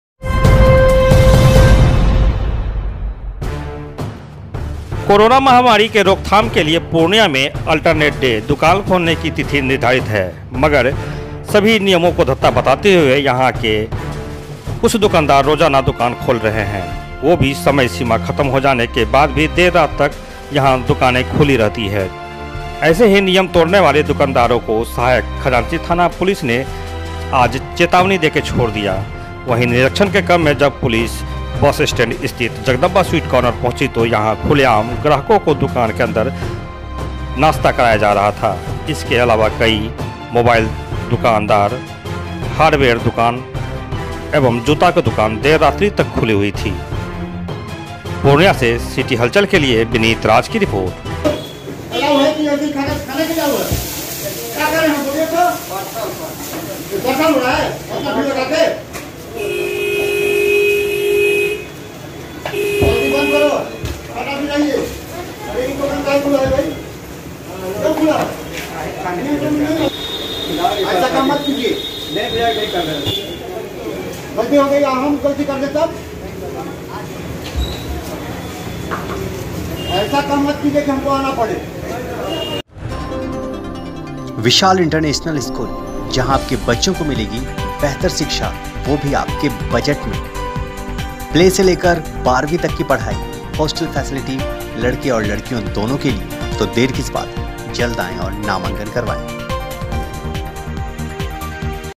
देर रात तक कई दुकान खुली मिली जगदम्बा स्वीट भी चेकिंग में पकड़ाया
कोरोना महामारी के रोकथाम के लिए पूर्णिया में अल्टरनेट डे दुकान खोलने की तिथि निर्धारित है। मगर सभी नियमों का धत्ता बताते हुए यहाँ रोजाना कई दुकानें खुल रही है। वो समय सीमा खत्म हो जाने के बाद भी देर रात तक खुली रहती है।
ऐसे ही नियम तोड़ने वाले दुकानदारों को सहायक खजांची थाना पुलिस ने चेतावनी देकर छोड़ दिया।
वहीं निरक्षण के क्रम में जब पुलिस जब बस स्टैंड स्थित जगदम्बा स्वीट कॉर्नर पहुँची तो यहाँ खुलेआम ग्राहकों को दुकान के अंदर खिलाया जा रहा रहा।
इसके अलावा कई मोबाइल दुकान हार्डवेयर जूता का दुकान देर रात्रि तक खुली हुई थी